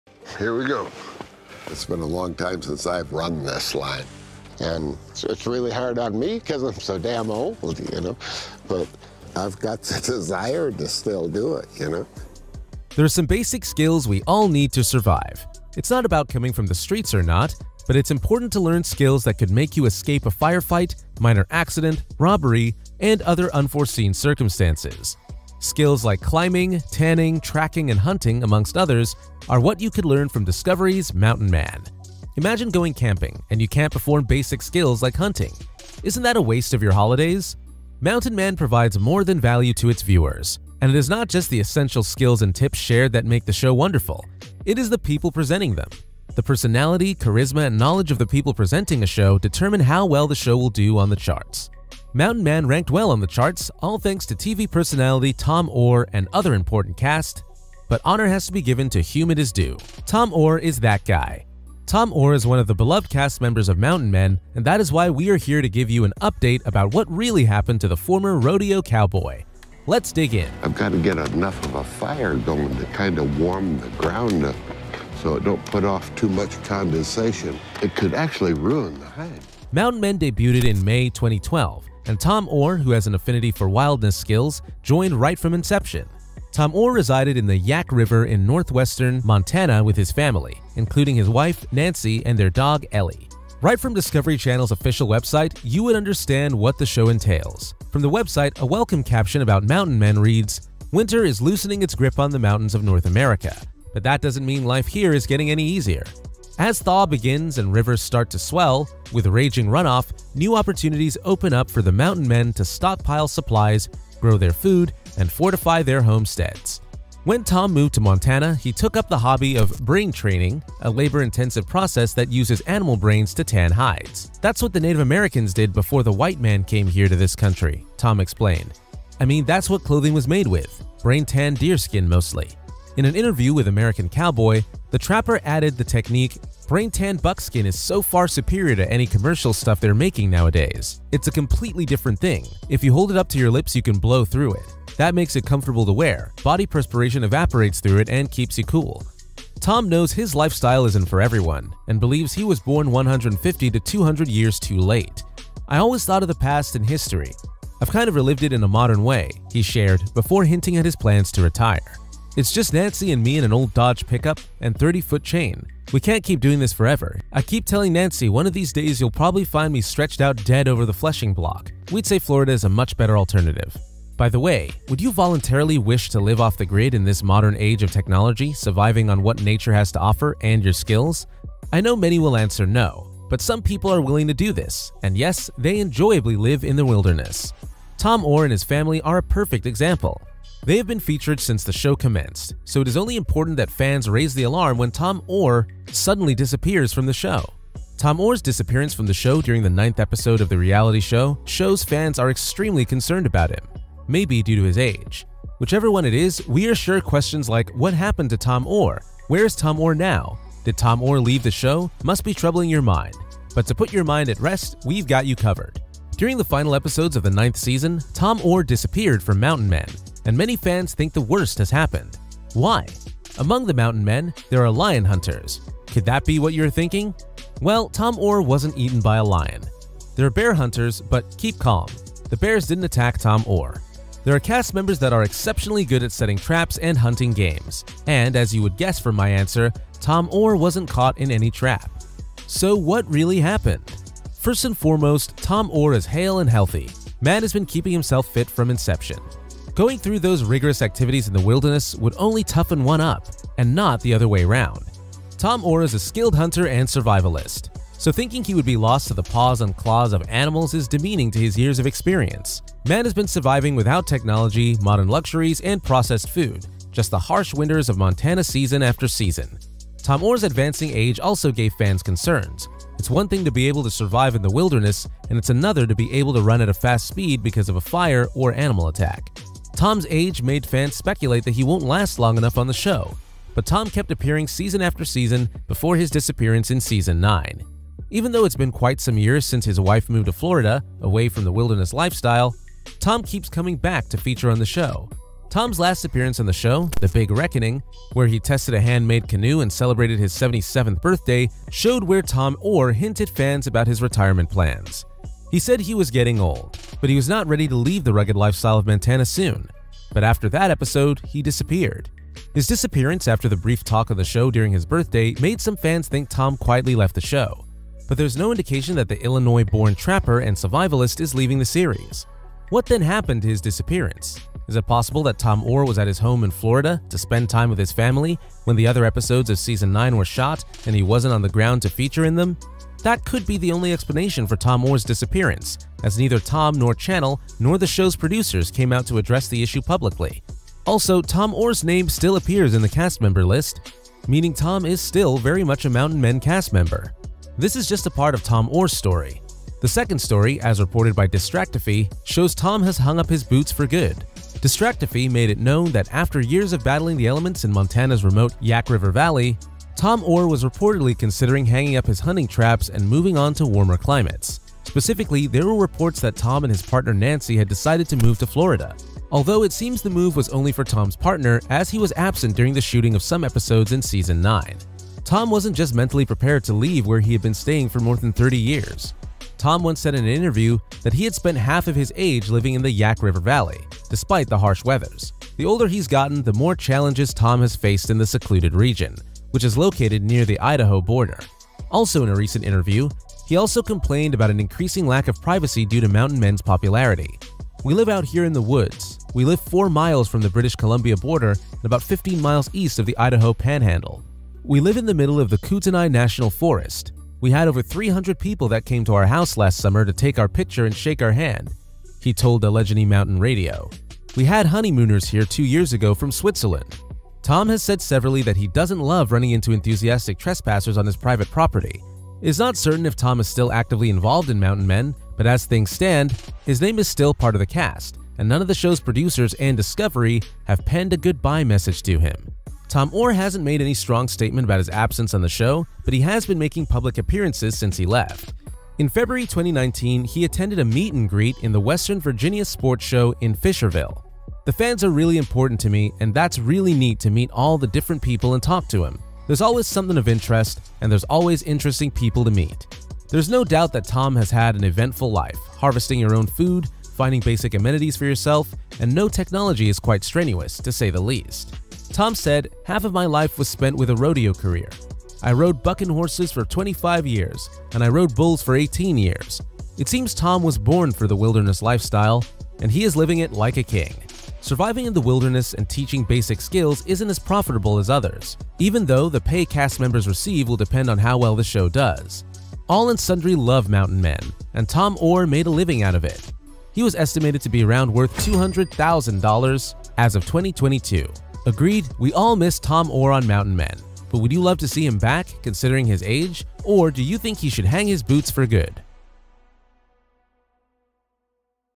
What Really Happened to Tom Oar From Mountain Men
There are some basic skills we all need to survive, it’s not about coming from the streets or not, but it’s important to learn skills that could make you escape a firefight, minor accident, robbery, and other unforeseen circumstances. Skills like climbing, tanning, tracking, and hunting, amongst others, are what you could learn from Discovery’s Mountain Men.

Imagine going camping, and you can’t perform basic skills like hunting. Isn’t that a waste of your holidays? Mountain Men provides more than value to its viewers, and it is not just the essential skills and tips shared that make the show wonderful — it is the people presenting them. The personality, charisma, and knowledge of the people presenting a show determine how well the show will do on the charts.

Mountain Men ranked well on the charts, all thanks to TV personality Tom Oars and other important cast, but honor has to be given to whom it is due; Tom Oar is that guy. Tom Oar is one of the beloved cast members of Mountain Men, and that is why we are here to give an update about what really happened to the former rodeo cowboy. Let’s dig in!